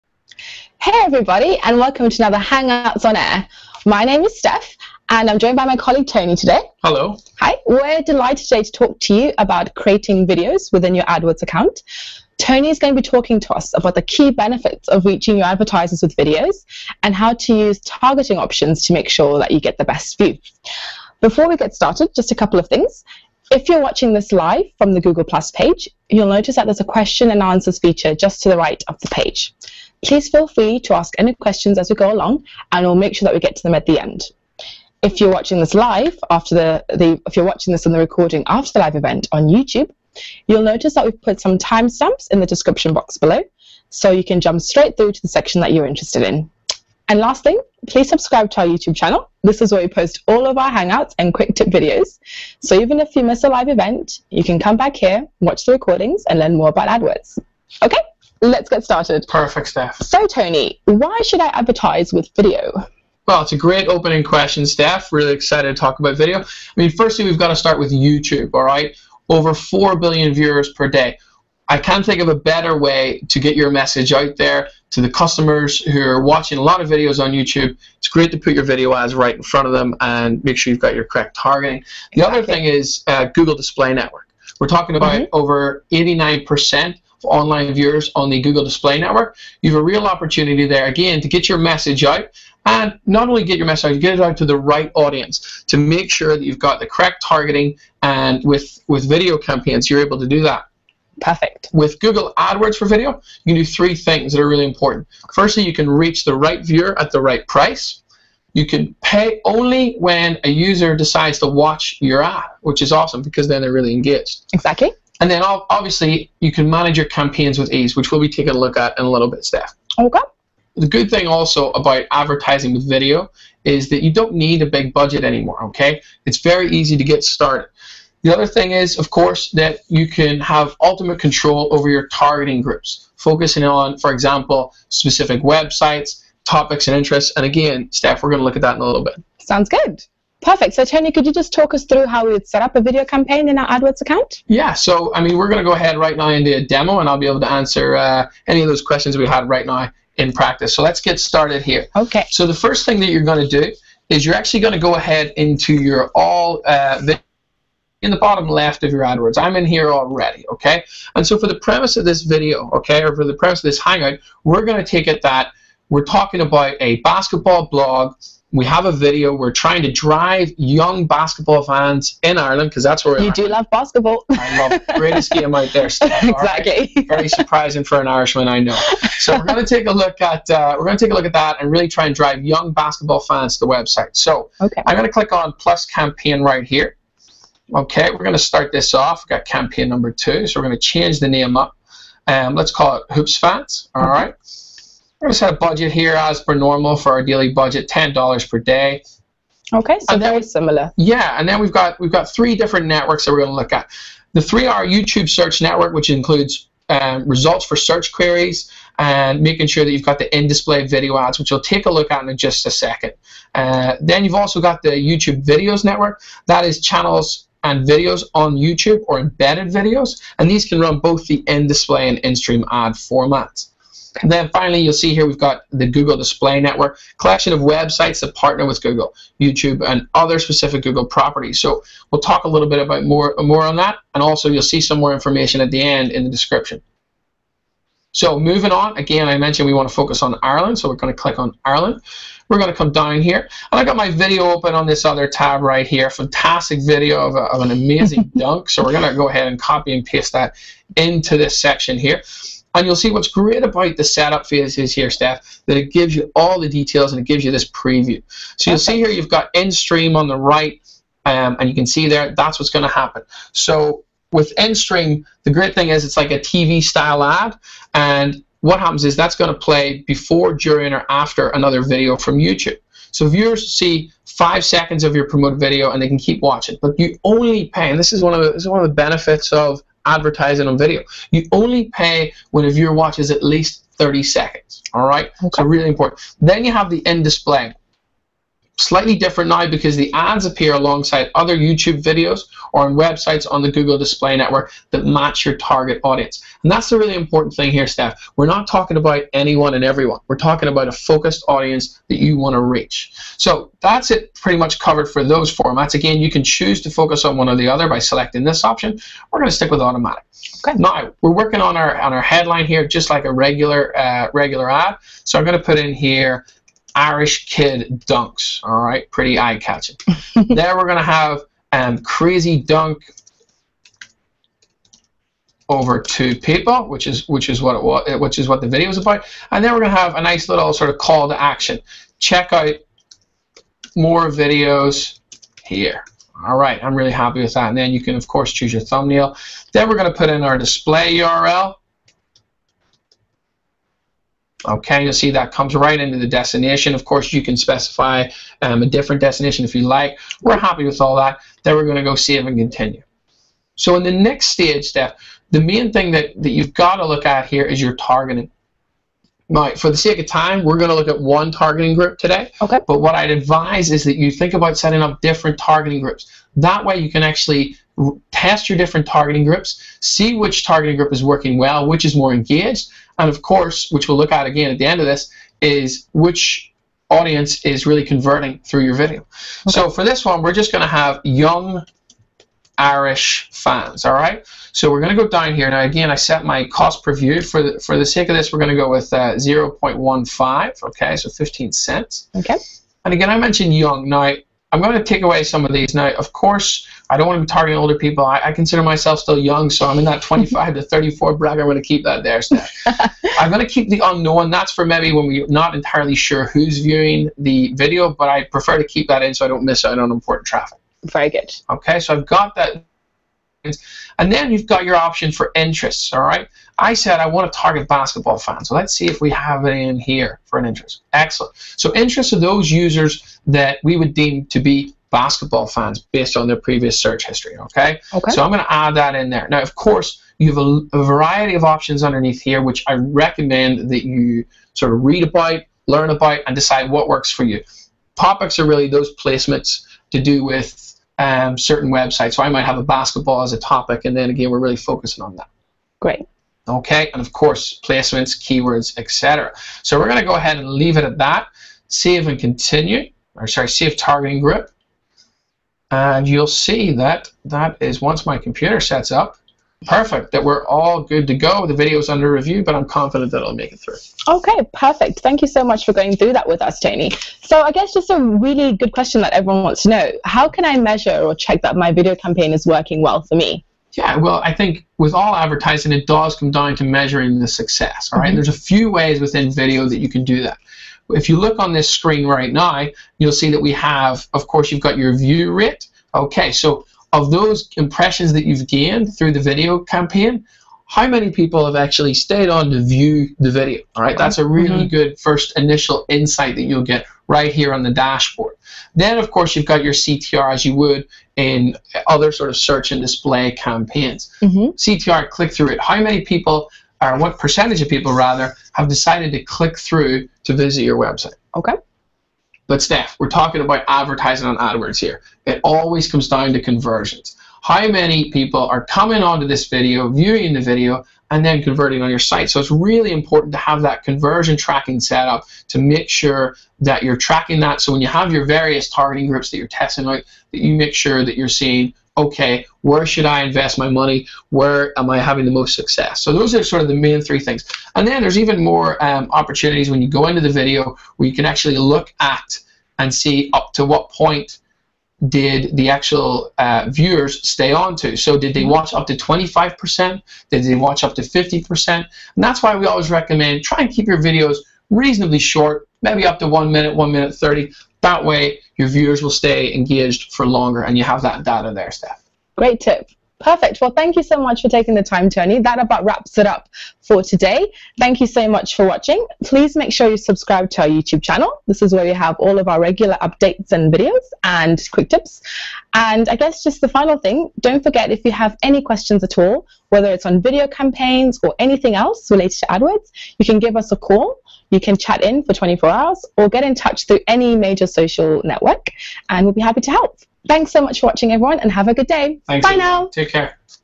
Campaign Starter Kit Series Part 5: Creating an AdWords for Video Campaign
1:09 - Why use AdWords for Video?
2:30 - How to set up a Video Campaign (Demo)
8:00 - Measuring the effectiveness of an AdWords for Video Campaign